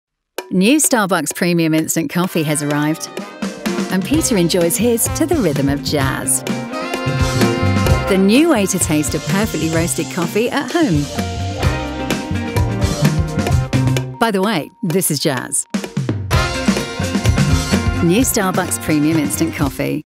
☕️ Starbucks y Nestlé lanzan nuevos productos para consumir en casa (1)
Campaña global de Nestlé por el lanzamiento de “Starbucks Premium Instant Coffee”.

“Starbucks Premium Instant Coffee” son una nueva gama de productos para consumir en casa,  que estarán disponibles en supermercados, así como a través de plataformas de venta online. La campaña ha sido creada por McCann Worldgroup. ¡Disfruta de esta campaña con aroma a café!